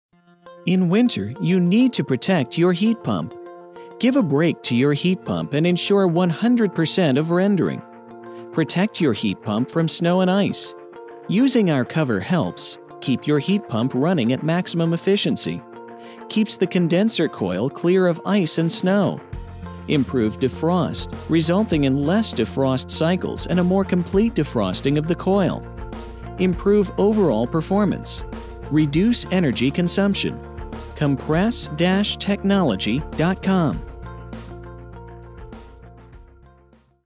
Heat Pump Cover made in Canada
DOES YOUR HEAT PUMP ICE UP ?

   Our heat pump cover will help with that! made of high impact, shatter resistant, UV protected polyethylene it's designed to provide the maximum protection for your heat pump in any weather conditions. The cover protects the condenser coil on all sides from direct contact with freezing rain, snow, roof runoff and hail as well as protects against falling ice that can dent the top and fold over the coil fins. The cover directs intake air from the bottom, side and threw louvers cut into the back panel to allow air to pass threw the coil precipitation free keeping the coil clear and extracting the maximum amount of heat from the air. While helping keep your heat pump free of ice and snow it also reduces defrost cycles and duration by holding heat inside the pump body while in defrost, this allows a for a full and complete defrosting of the coil for maximum heat exchange.

   The cover is easily assembled in about 5 minutes using only a Phillips screw driver and installs on the pump in less than 1 minute. It is attached to the heat pump only ( not your house) using 2 cam buckle straps that wrap around the body and pull to a tight fit with little effort. It's a universal fit, the top and back panel slide into the side panel allow it to fit most 12 - 18 and 24 000 Btu heat pumps with no modifications needed. All contact points inside the cover are lined with protective foam to prevent chaffing and vibration as well as allows for impact absorption from falling ice.

Proudly made in Canada
At Compress Technology We've spent 2 years designing and developing this cover to be able to withstand the worst possible Atlantic Canadian ice storm
It assembles and installs in 5 minutes with stainless steel hardware and attach on your pump using Heavy-Duty cam buckle straps.
Made from High impact polyethylene plastic lined with foam,it won't chafe or vibrate and will no crack or break from falling ice in temperatures down to -46 degrees Celsius